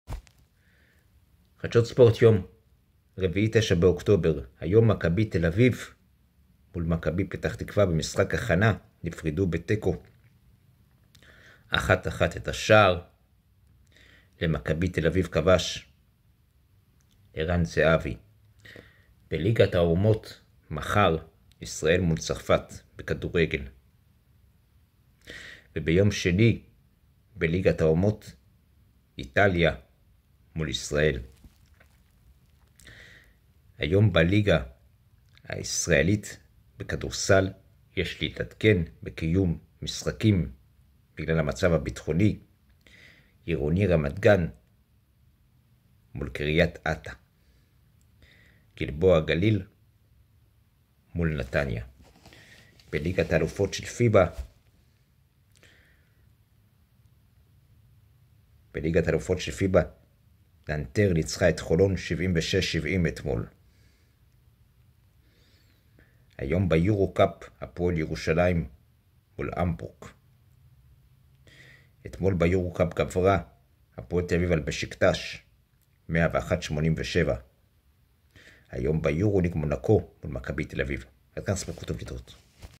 יום 3469 day -חדשות ספורט רביעי 09.10.24
תוספת
עפולה 1 עכו 1
מונאקו 85 מכבי תא 79
הפועל ירושלים 89 המבורג 74
רמת גן 93 קרית אתא 75
גלבוע גליל 100 נתניה 67